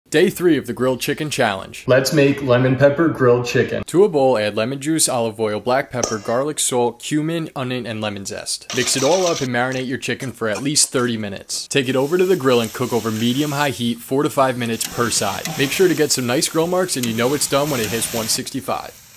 How To Make Lemon Pepper Grilled Chicken: Healthy And Delicious Chicken
Healthy Lemon Pepper Grilled Chicken

Per Thigh  — Servings 4
- 233 Calories
- 22g Protein
- 4g Carbs
- 15g Fat

Ingredients
1 LB Organic Chicken Thigh
1/4 Cup Lemon Juice
2 TBSP Olive Oil
1/2 TSP Granulated Onion
3 Cloves Garlic, Minced
1/4 TSP Cumin
1 TBSP Lemon Zest
1/2 TSP Salt & Pepper, To Taste

Recipe
1. Add The Lemon Juice, Olive Oil, Onion, Garlic, Cumin, Lemon Zest, Salt And Pepper To A Bowl Or Dish. Mix Well.
2. Add In Your Chicken. Coat Every Last Inch Of The Chicken In The Marinade And Let It Sit For At Least 30 Min In The Fridge.
3. Heat Your Grill To Med-High. Cook Over Direct Heat With The Lid closed 4-5 Min Per Side.
4. Rest The Chicken For 5 Min And Enjoy!!!